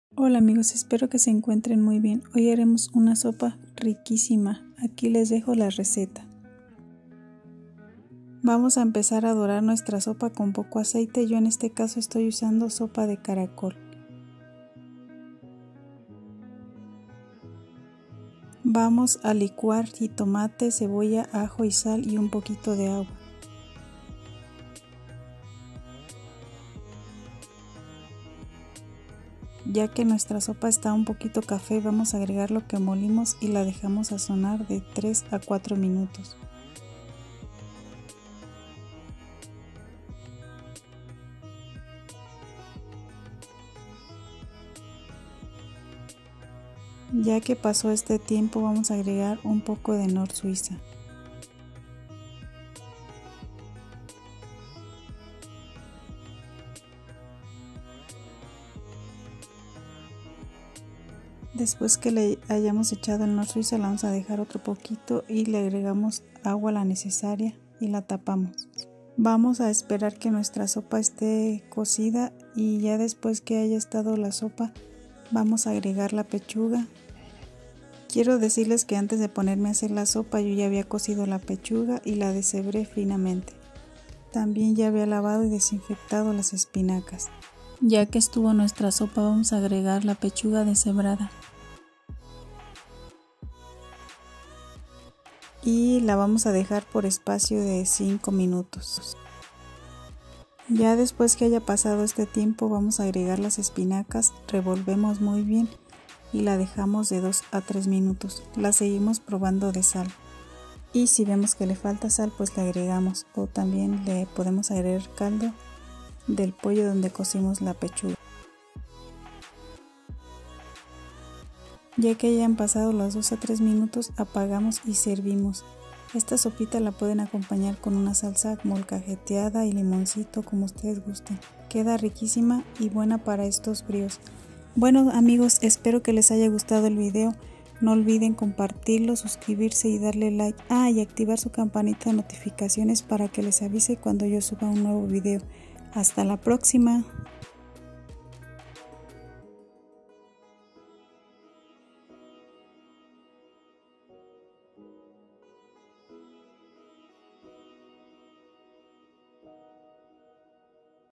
Como Hacer Sopa con pollo y espinacas
Hola amigos🤗 hoy les traigo, esta receta de sopa con pollo y espinacas queda riquisima, espero se animen hacer esta sopa con pollo y espinacas. 😋

Ingredientes
Una pechuga de pollo chica
Cebolla
3 ajos
Sal al gusto
Agua
(Esto es ara cocer la pechuga)

Sopa de caracol
4 jitomate medianos
1/4 de cebolla
1 ajo chico
Knorsuiza
Sal al gusto  agua la necesaria
Espinaca al gusto

Músico: ASHUTOSH